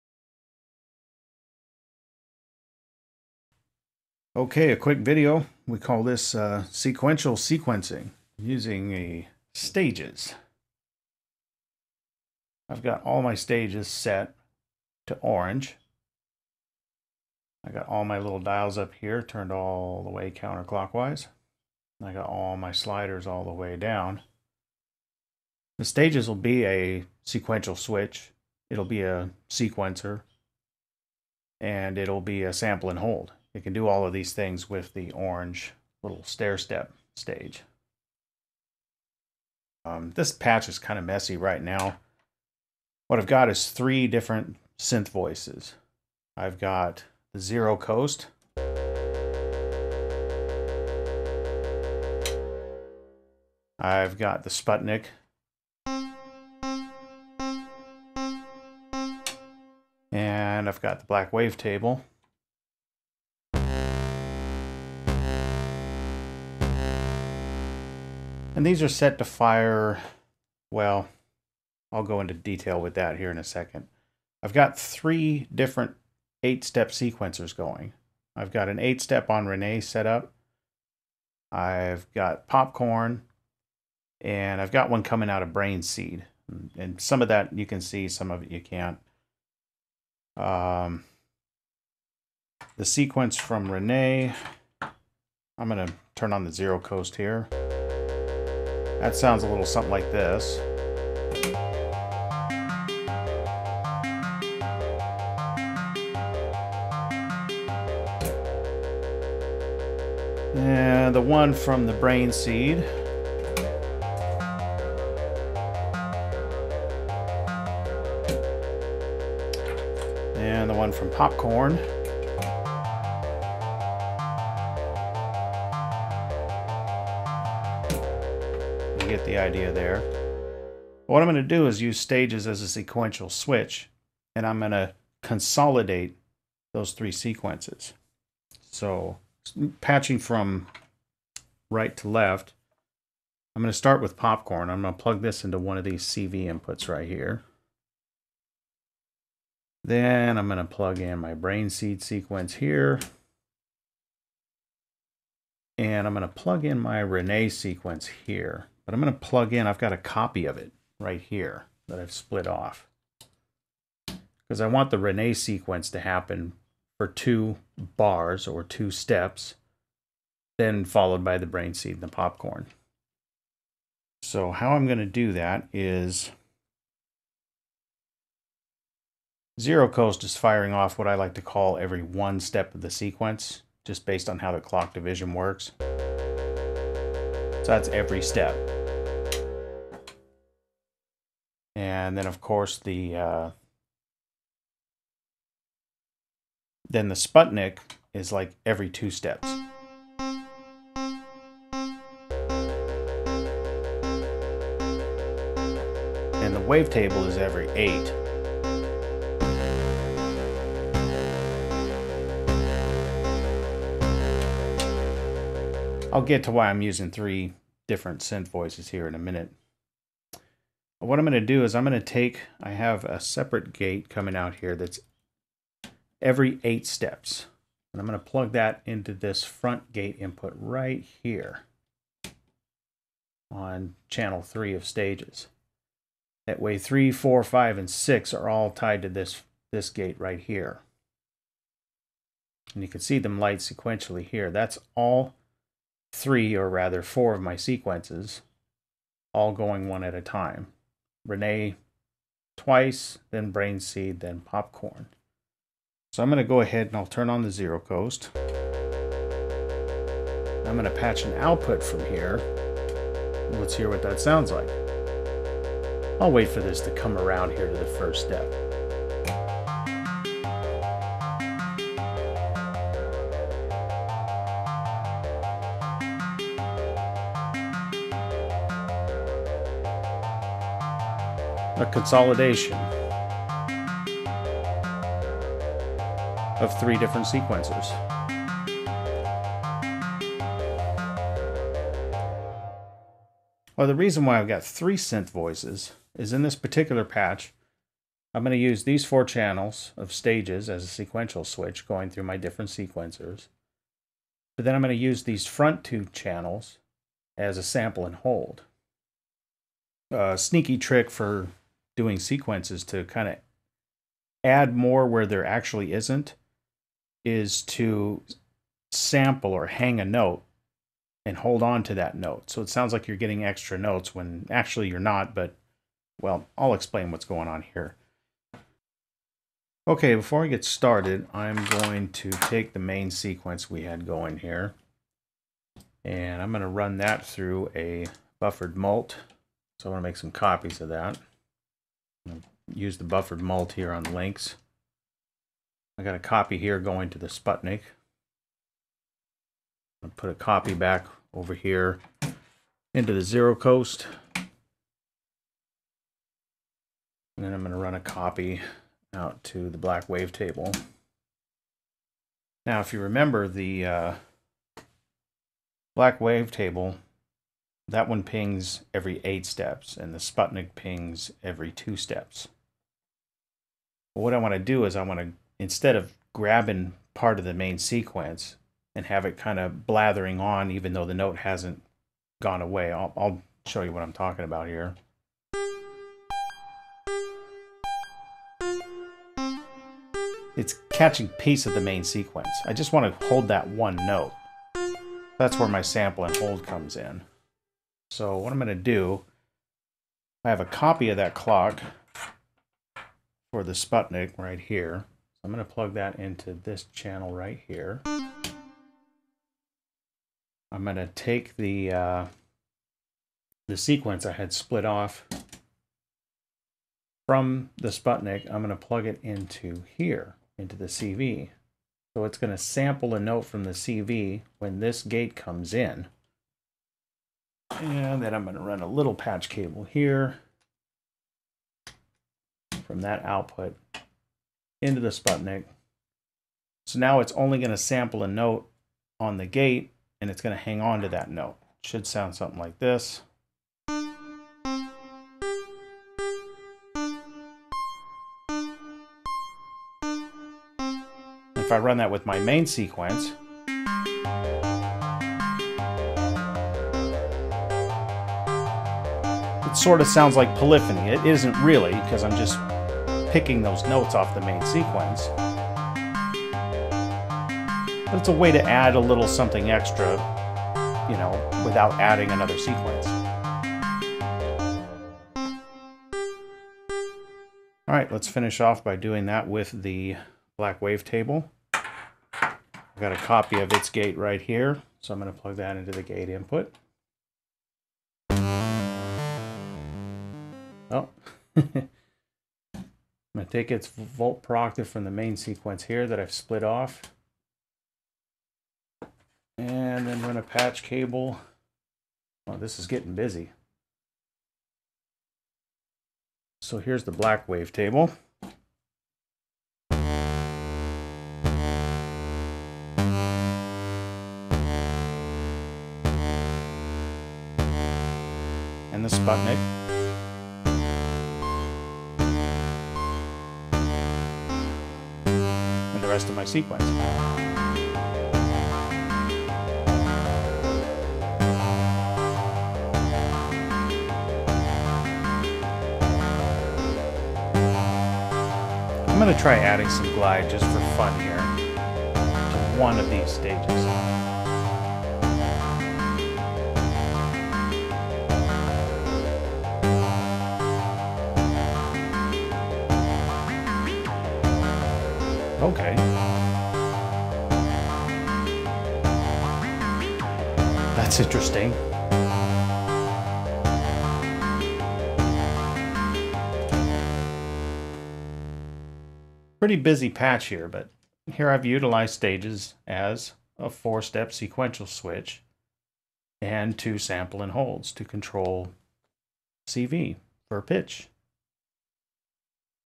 Mutable Stages - Sequential Sequencing
Using Stages as a sequential switch and sample + hold to manipulate CV sequences.